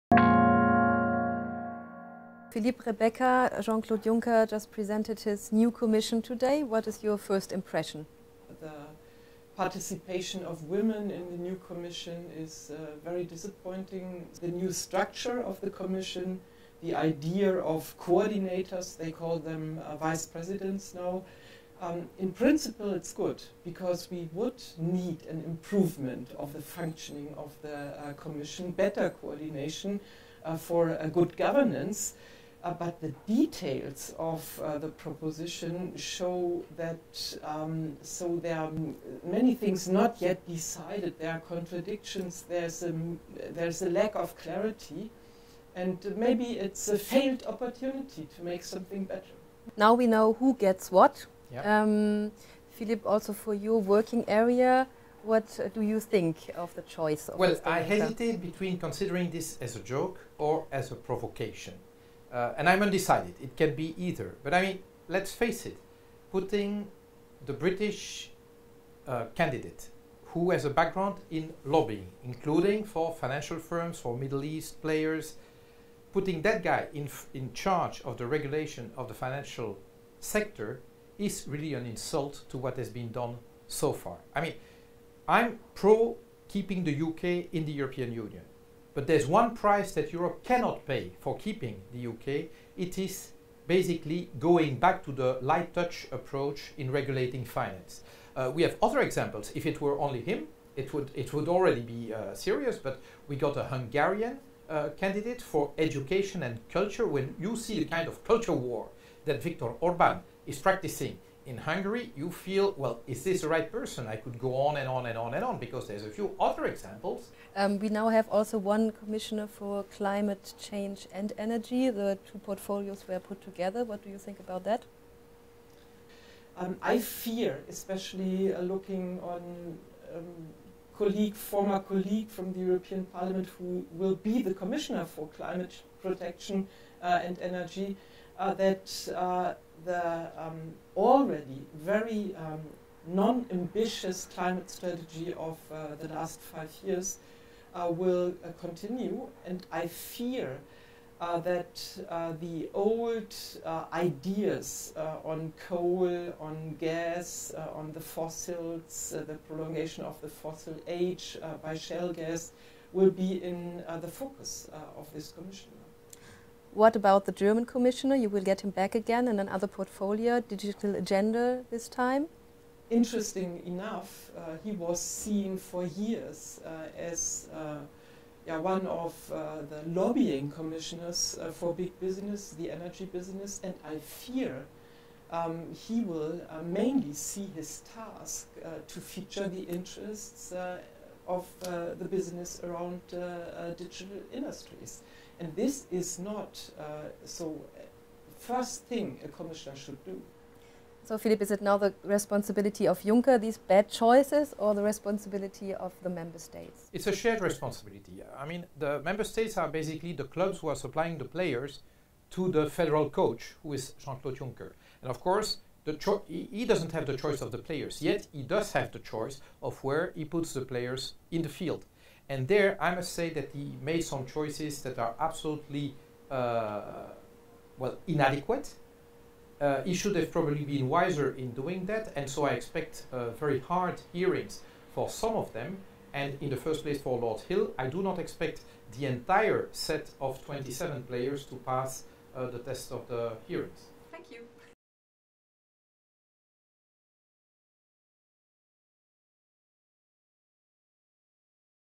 Team Juncker: initial Green reaction to the proposed EU Commission
Greens/EFA co-presidents Philippe Lamberts and Rebecca Harms give their initial reaction to the proposed new European Commission presented by Jean Claude Juncker.